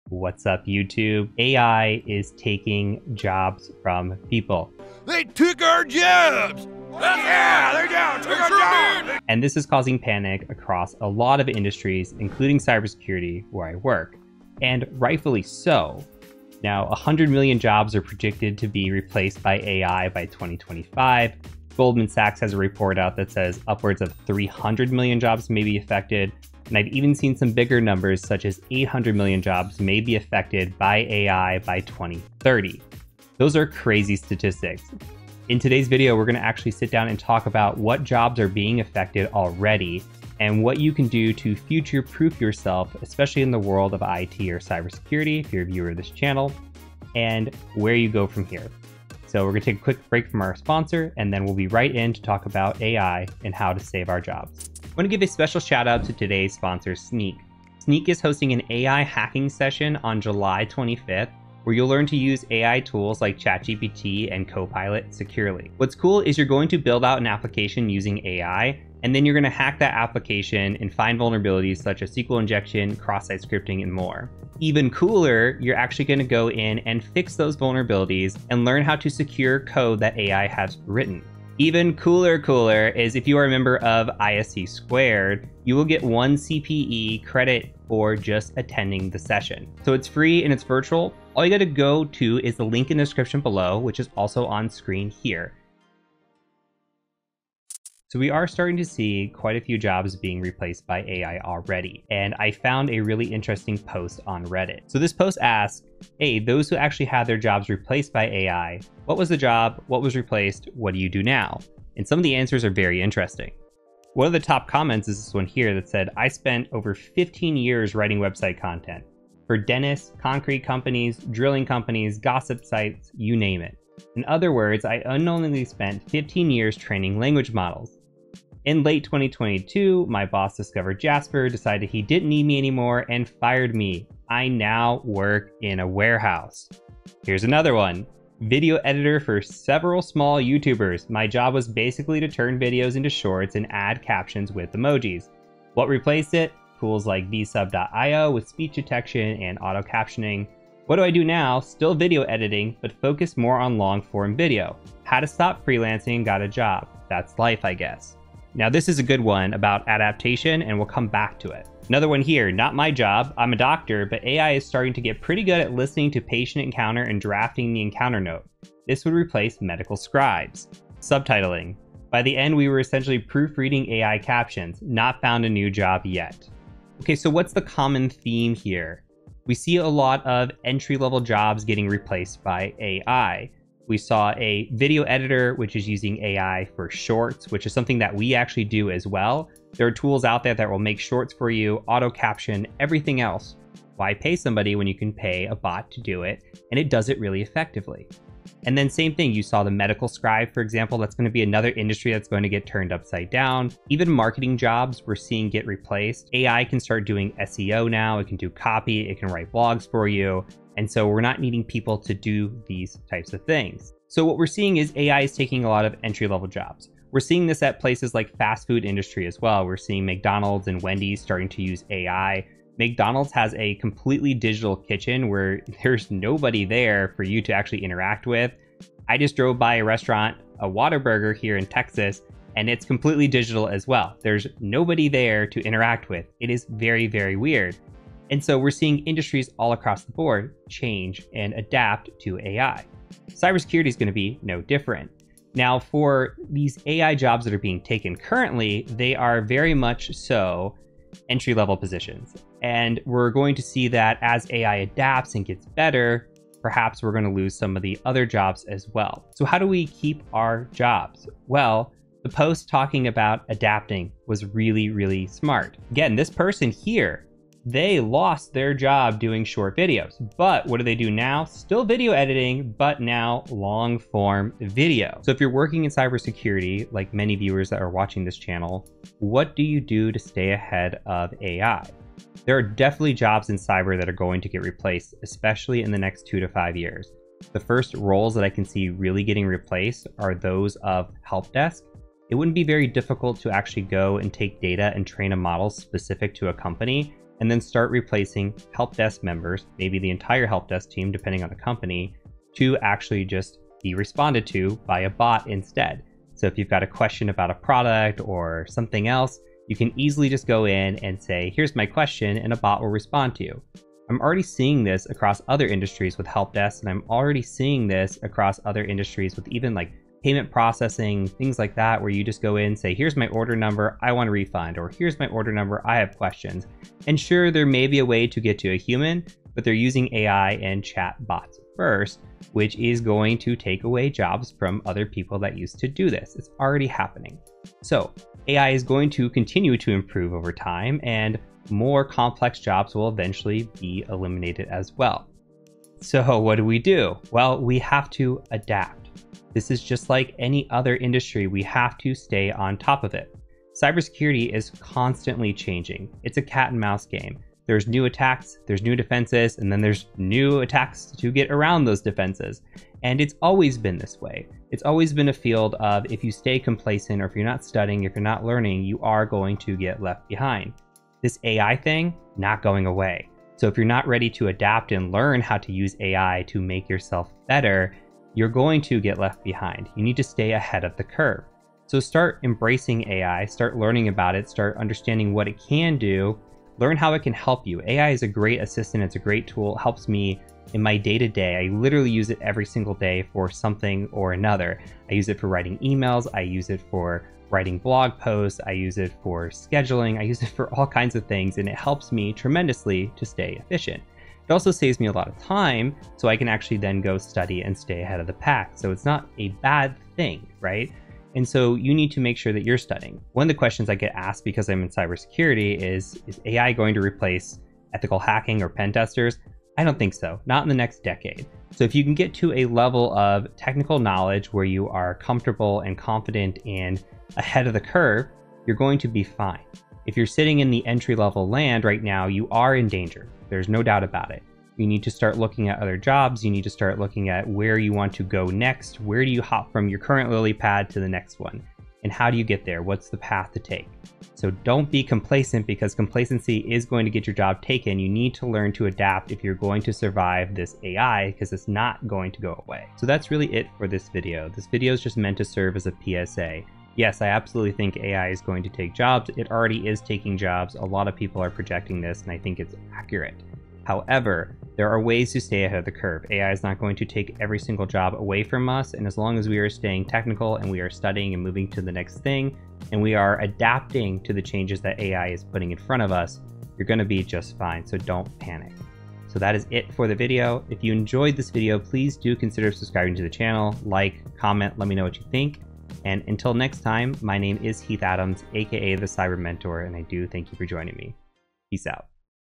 Could AI Take Our Jobs?
Check out Snyk's AI hacking workshop on July 25th.

Could AI eventually take over cybersecurity jobs? Heath shares some insights on this hot topic in this video. Learn what artificial intelligence in the security space could look like in the very near future!